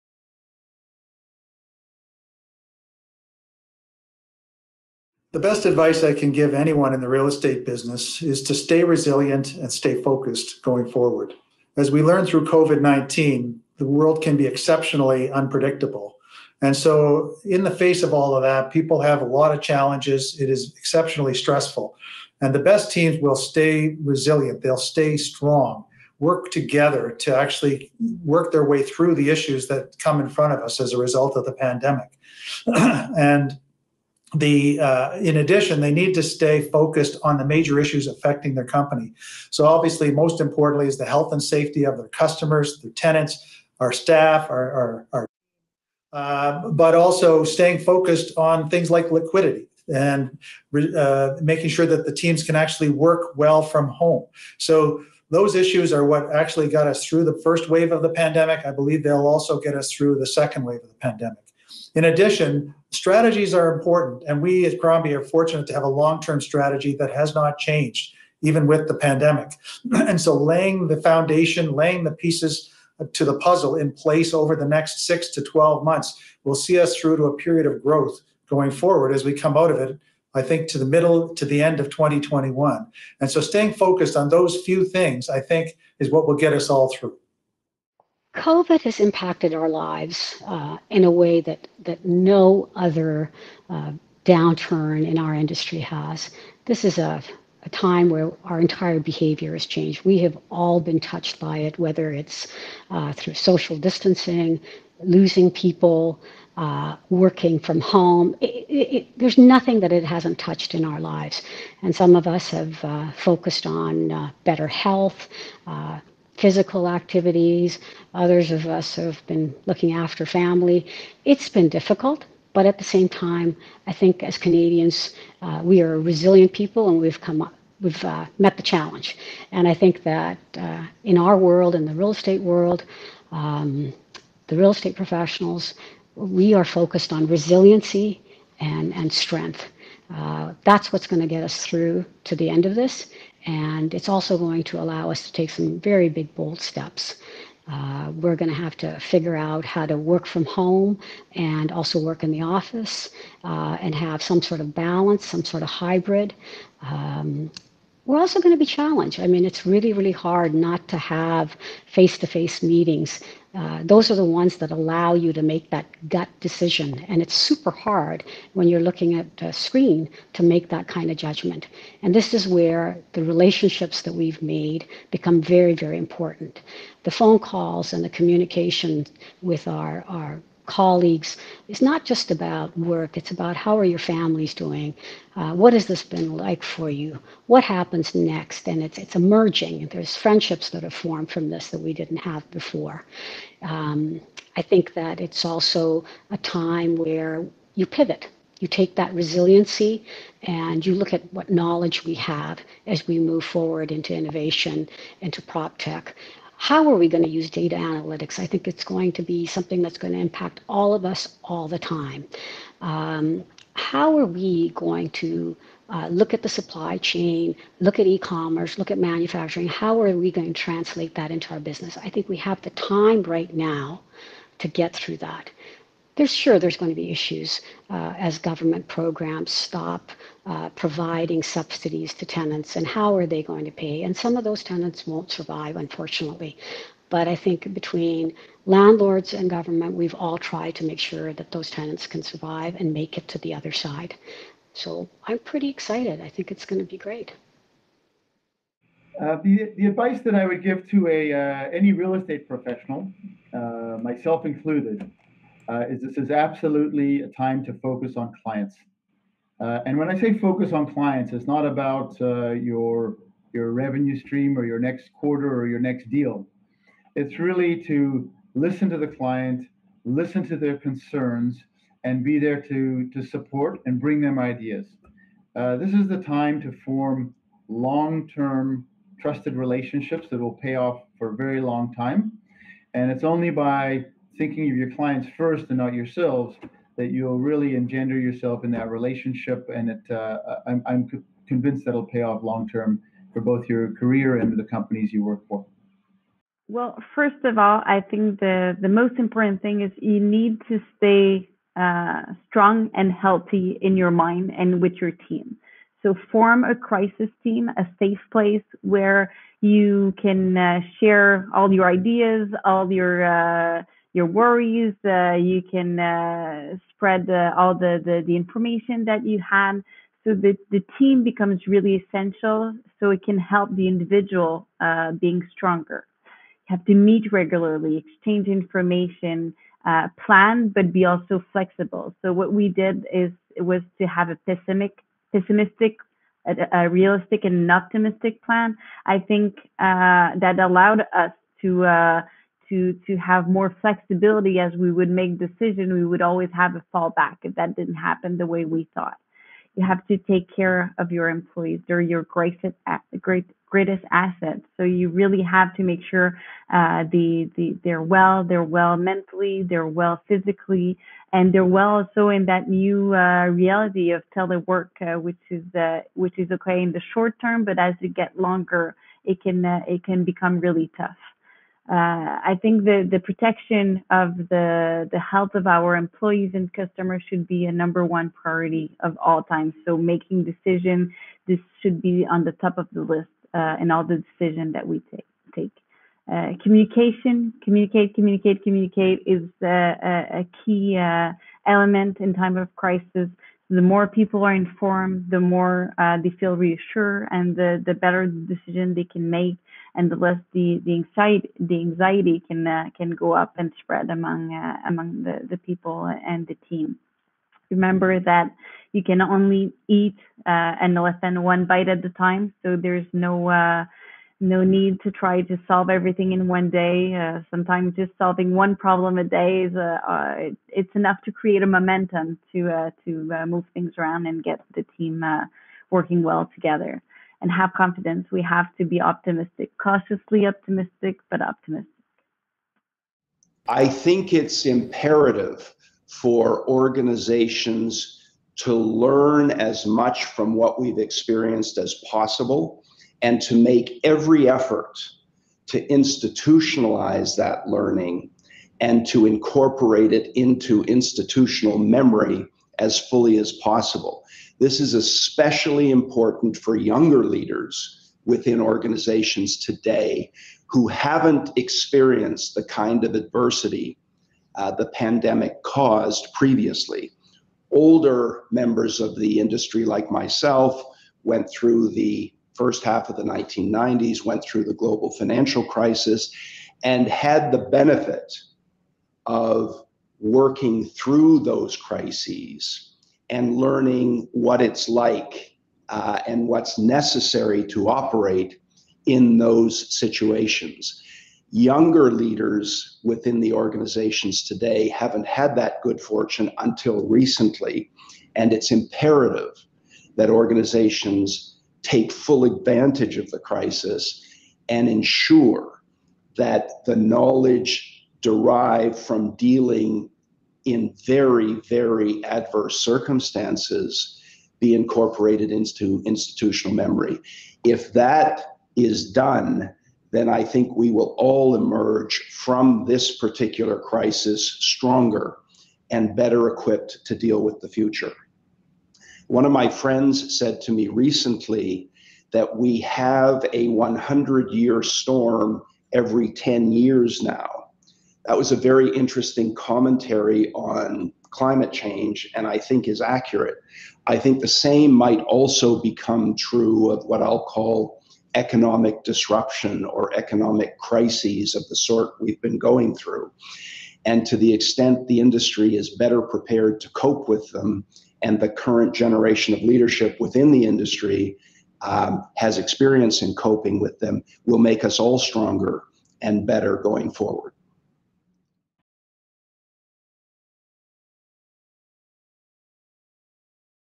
Given the impact of COVID-19, what advice would you offer Canadian real estate professionals?
Recorded at the virtual Toronto Real Estate Forum 2020